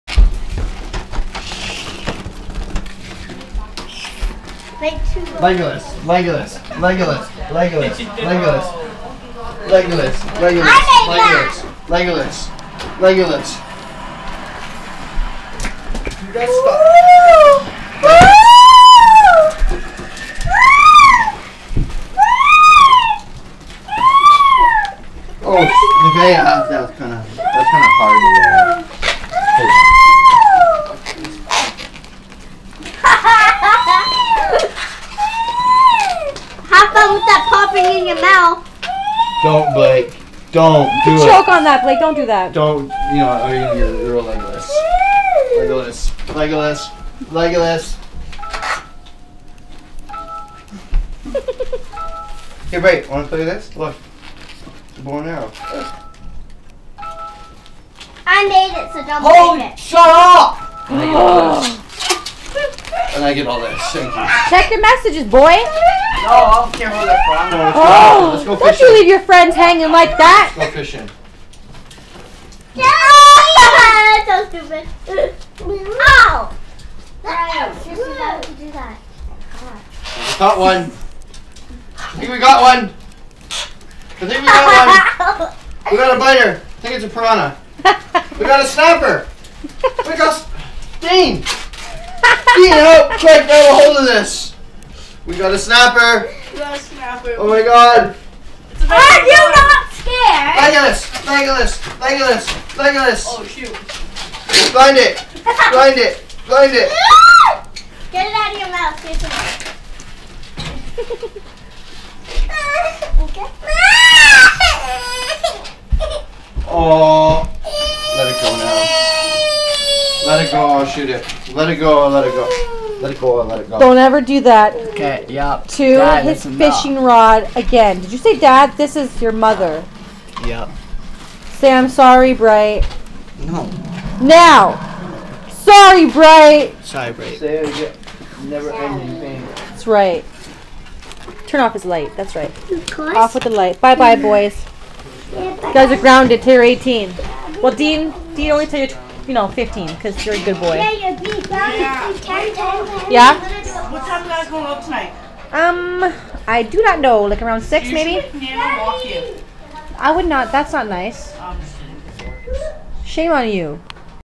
fun with balloons!
Daddy Reckless made some fun things with balloons and the kids are messing around with them..:)